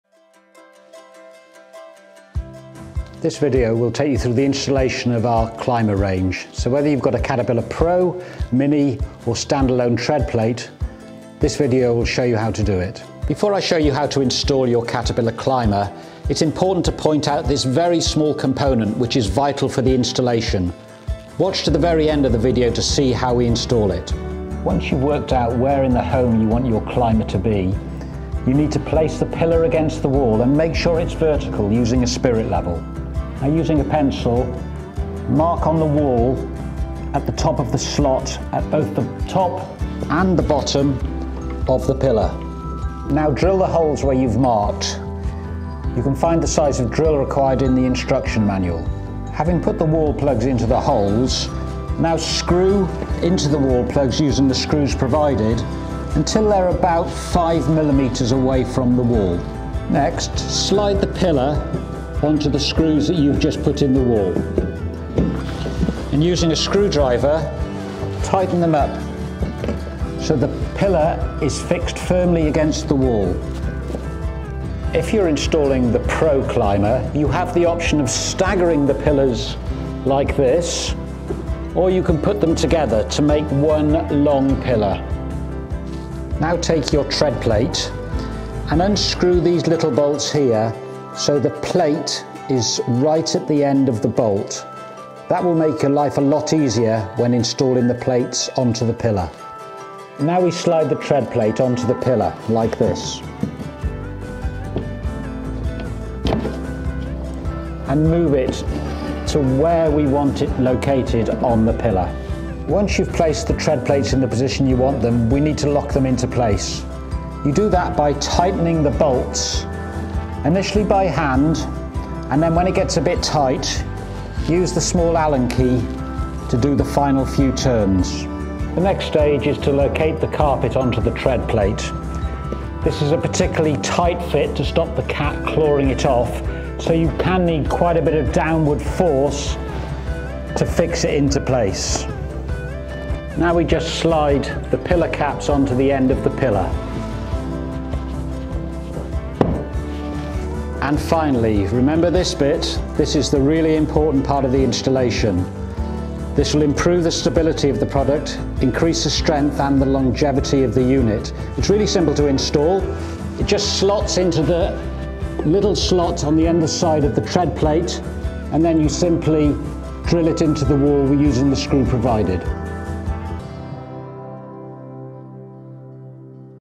Catipilla Installation Video | Climber Range (Pro, Mini, Tread Plate Step)
A short video explaining how to install the Catipilla Pro, Mini & Tread Plate Step. Note that this video runs through the installation of the Pro Cat Climber but the process is identical for the Mini and Tread Plate Step units.

Each unit comes with a step-by-step installation guide and screws. Standard wall plugs are also supplied in every order. Please note that specialist wall plugs are required for plasterboard walls.